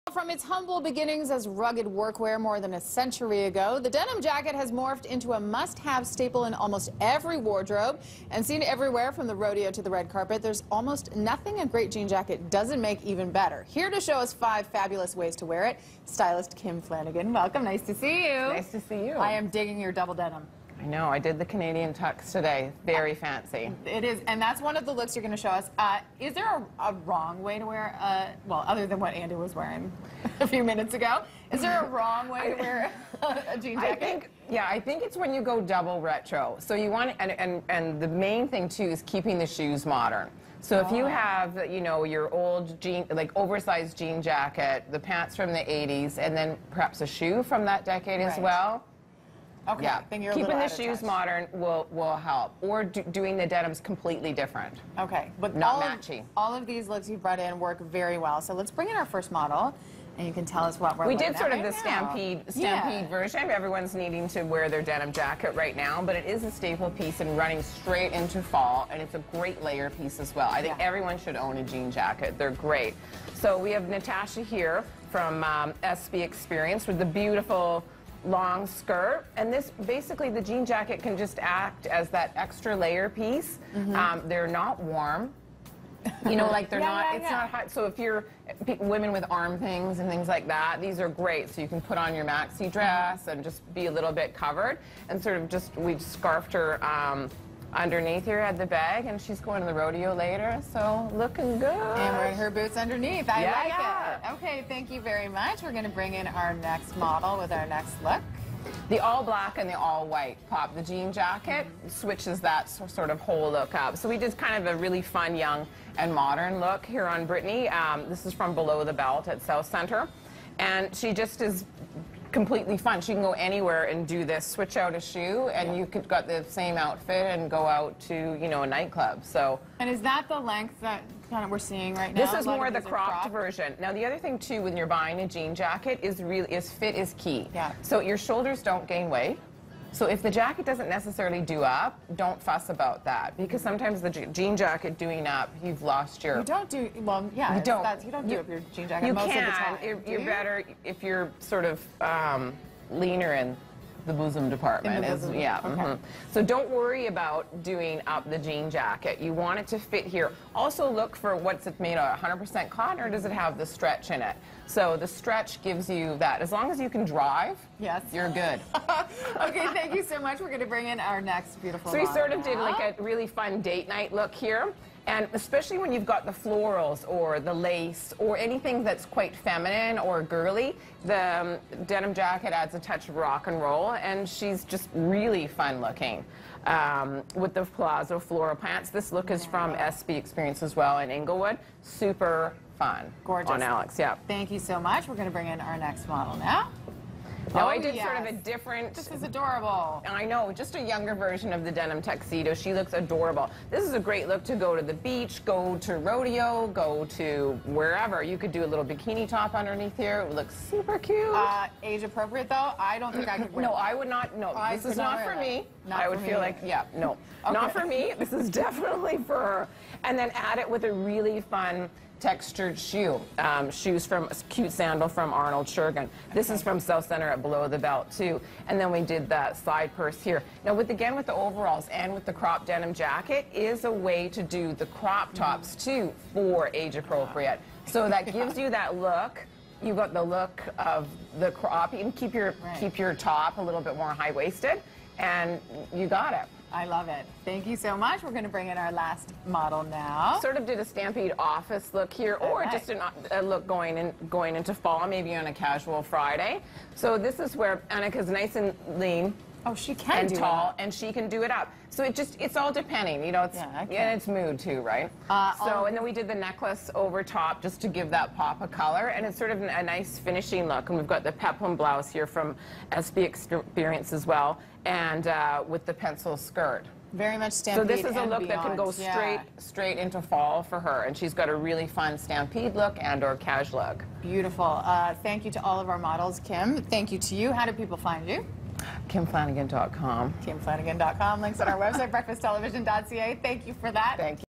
Jean Jacket Done 5 Ways - July 9th
One denim jacket -- five looks! Stylist Kim Flanagan has the tricks to work that closet favourite, for any age or occasion!